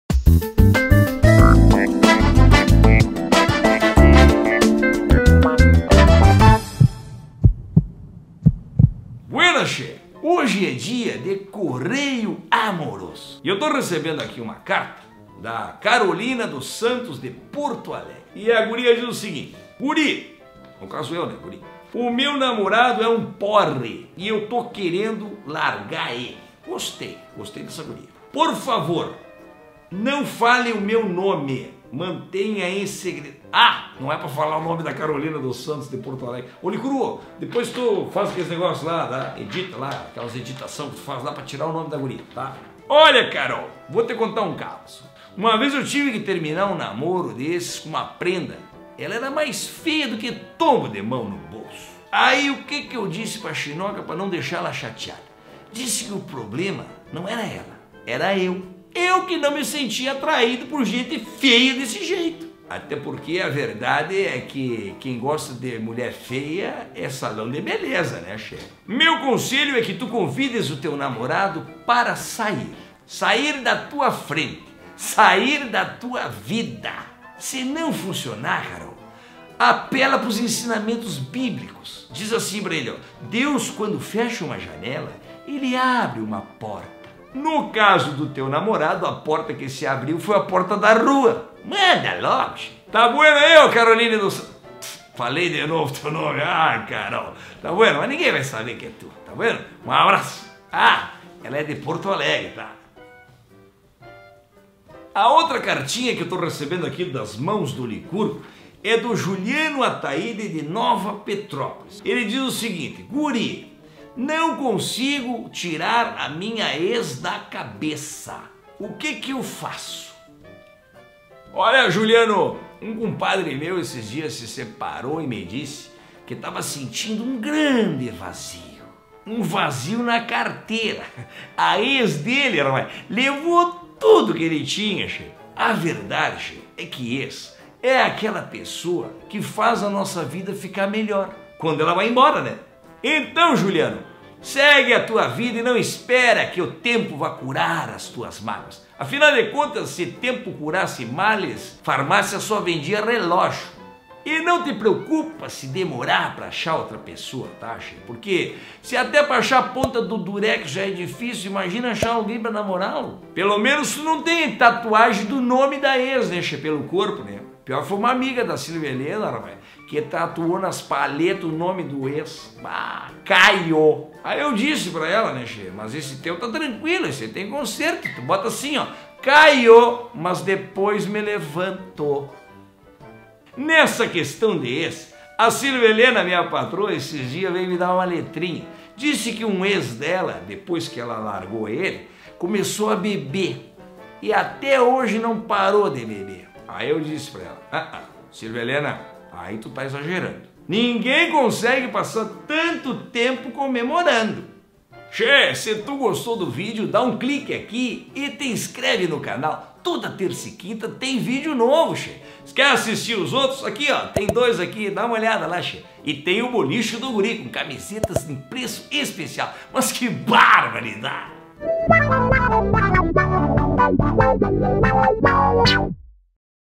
Meu namorado é um porre | Correio Amoroso #4 - Guri de Uruguaiana
Guri de Uruguaiana responde às dúvidas amorosas dos internautas.
O humorista gaúcho Jair Kobe é o criador do personagem Guri de Uruguaiana.